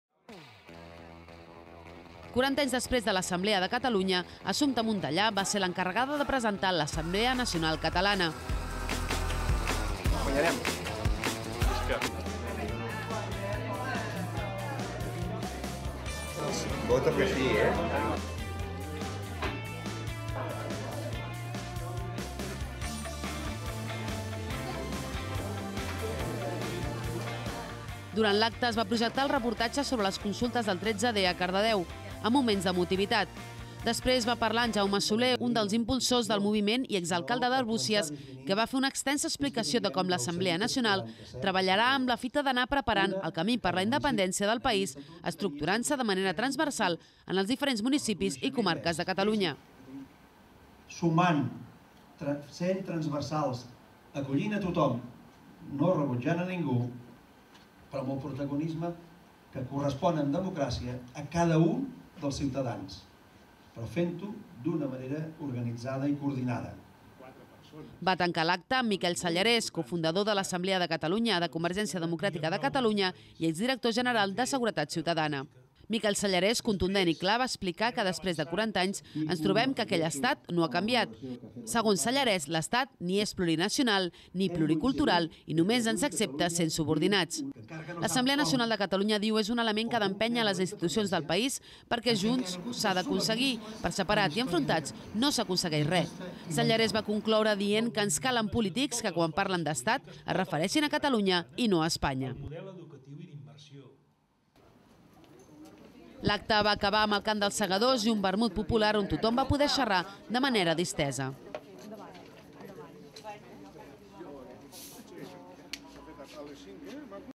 Refundació de l'Assemblea Nacional Catalana
Ahir diumenge cap a les 12 del migdia a l'Esbarjo, va haver-hi la presentació de l'assemblea nacional catalana  a Cardedeu amb la participació de Miquel Sellarés i Jaume Soler.